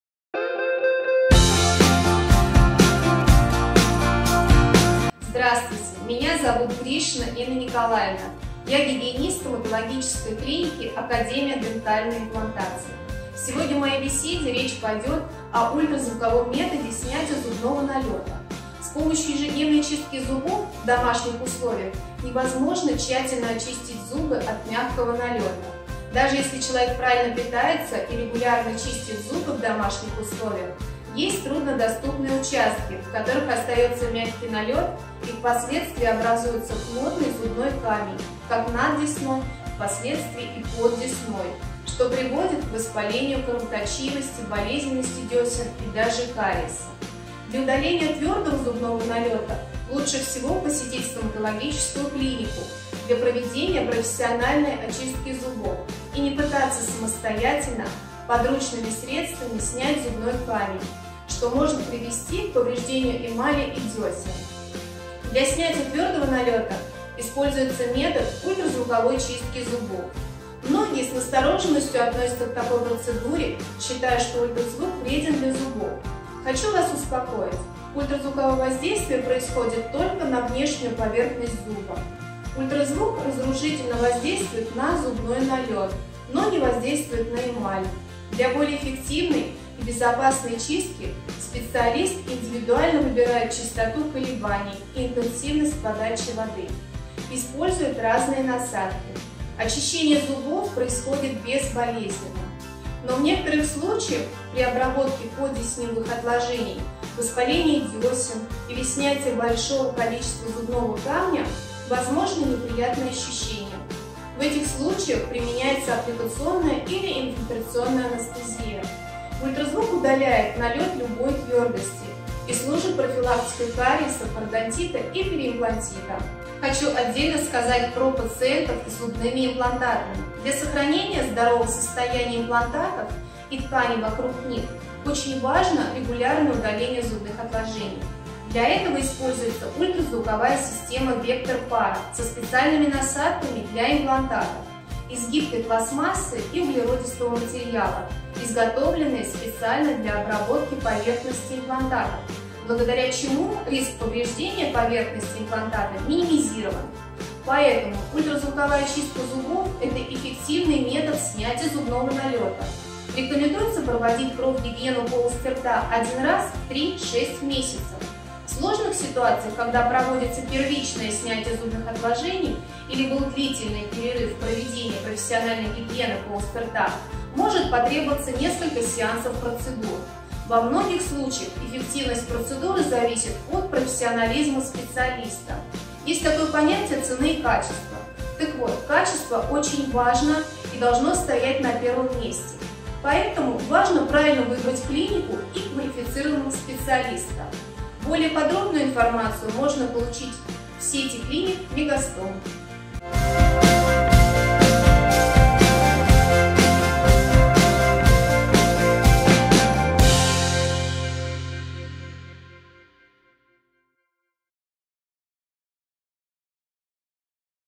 Консультация у гигиениста_Мегастом
Ведущий стоматолог-гигиенист клиники "Академия Дентальной Имплантации"  Гришина Инна Николаевна рассказала о гигиене полости рта и консультации на приеме у врача-гигиениста.